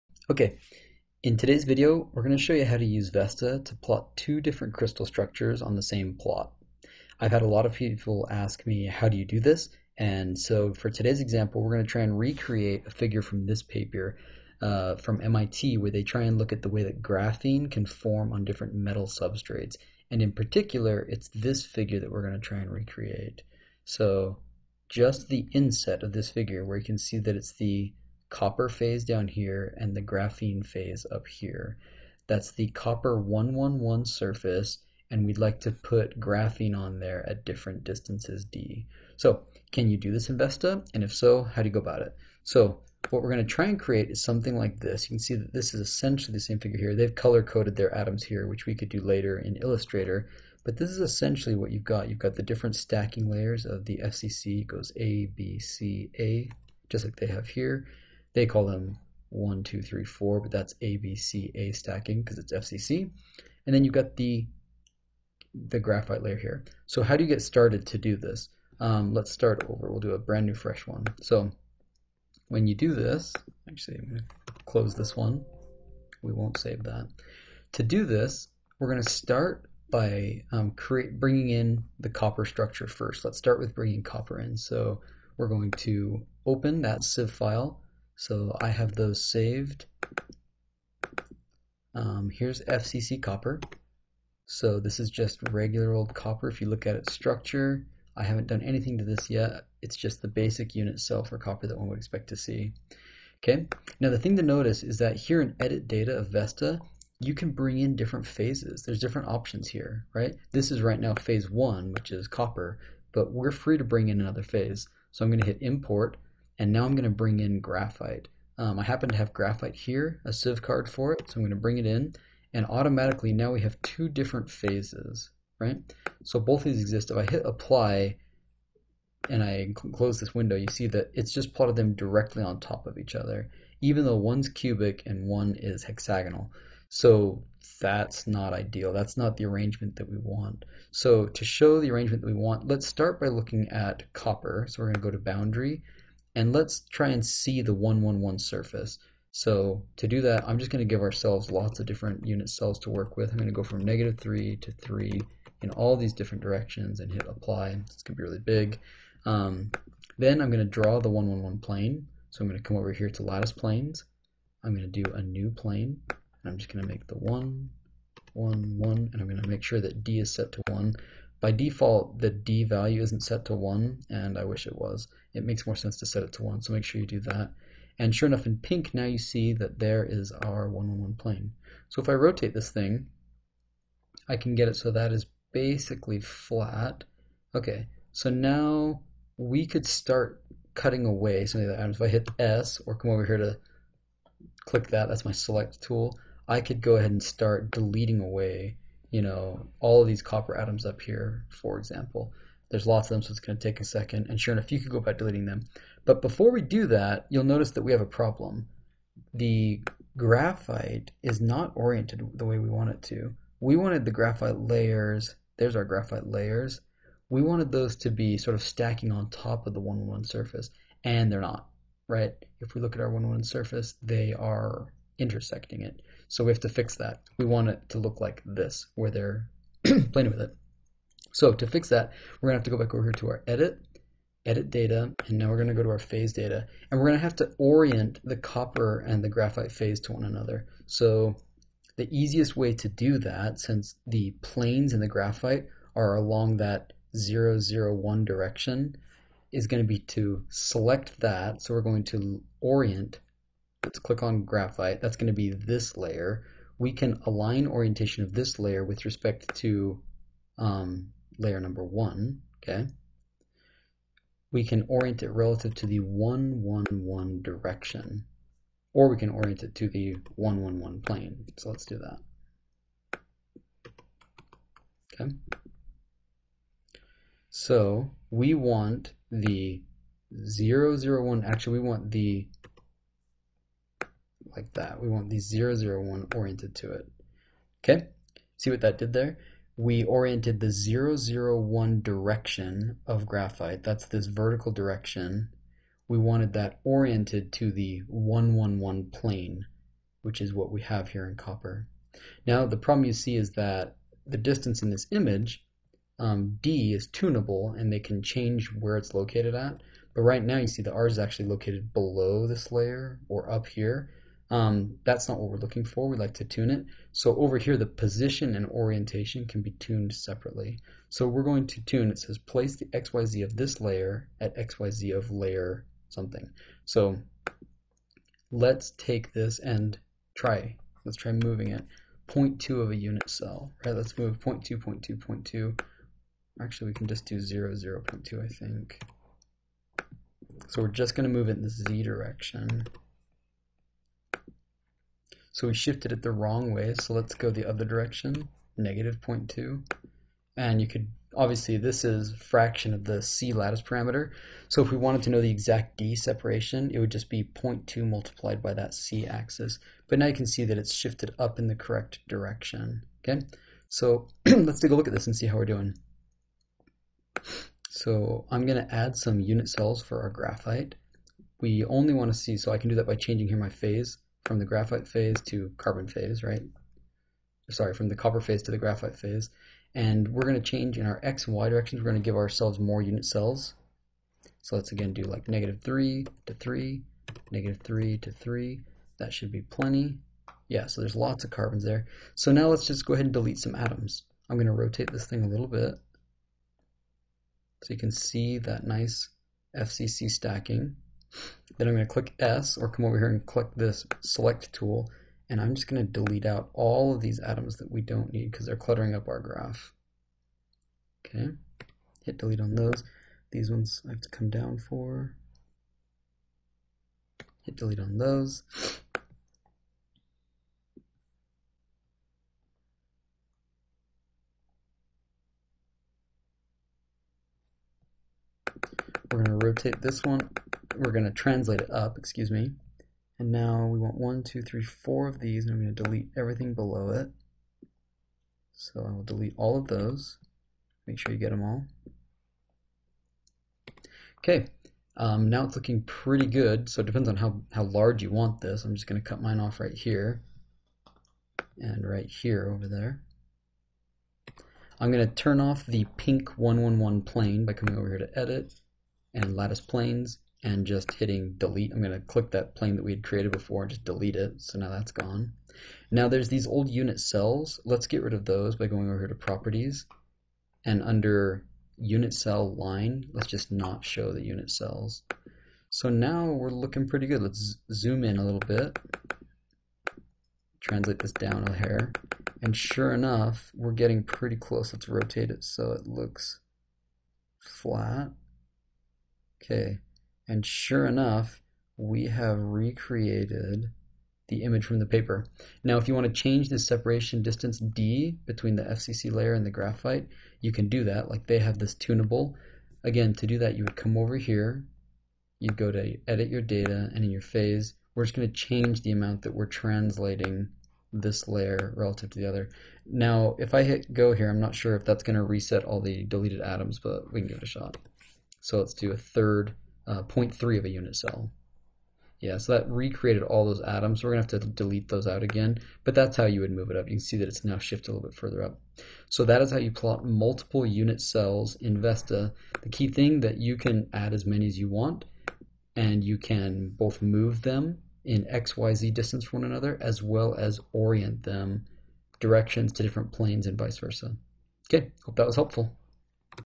How to plot multiple crystal structure phases in VESTA (tutorial)
Many times we want to plot more than one crystal structure next to another. VESTA allows us to do so by importing multiple phases and then changing their position and orientation one to another. In this video, I show how to plot a graphene layer that is located on a Cu (111) surface.